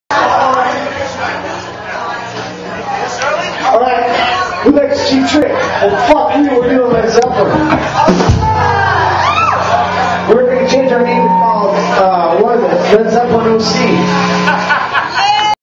Led Zeppelin OC
Disguster live @ the Slidebar in Fullerton, California. Recorded on July 1, 2010.